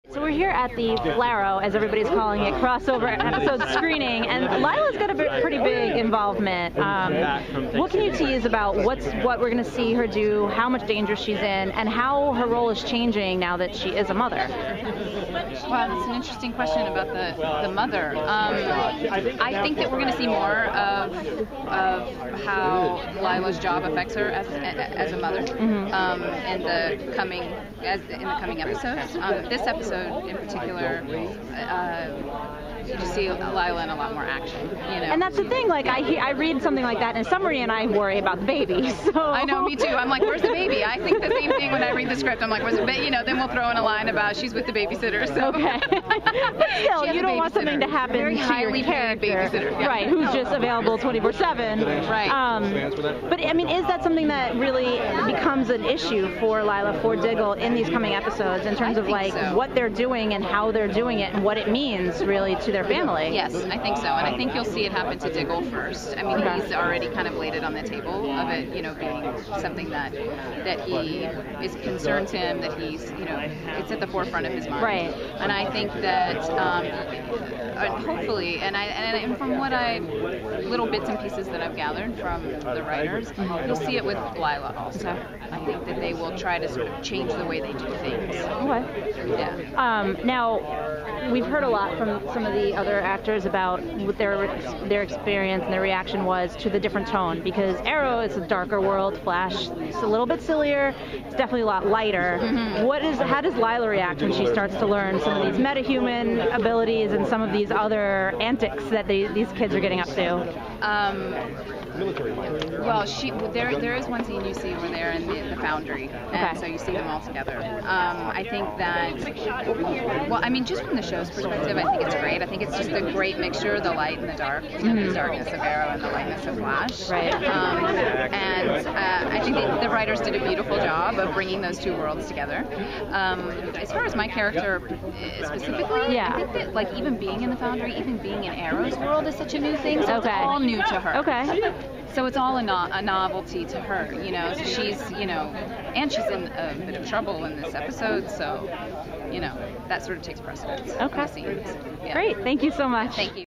Audrey Marie Anderson talks 'Arrow'/'The Flash' crossover
I caught up with Audrey Marie Anderson at a special event for 'Arrow' and 'The Flash's' two-hour/two-night crossover to talk about Lyla in danger and how being a mother affects her character and Diggle's.